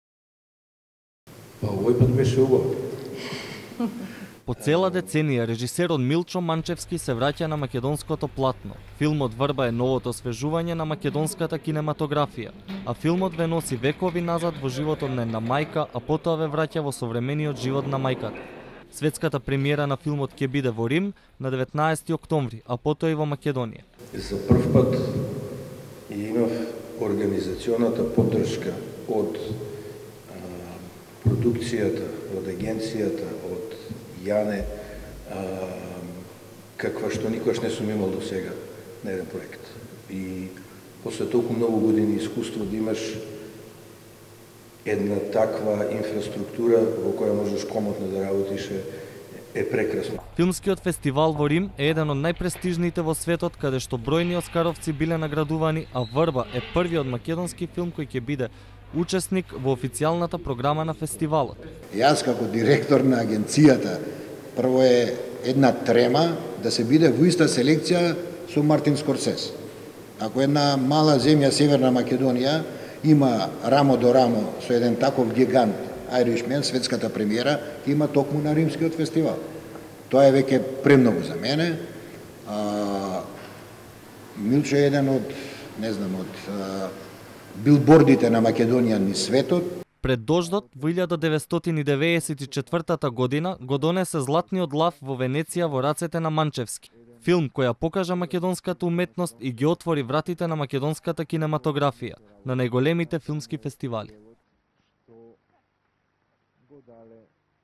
„Врба“ на Манчевски со светска премиера на филмскиот фестивал во Рим
По цела деценија режисерот Милчо Манечвски се враќа на македонското платно.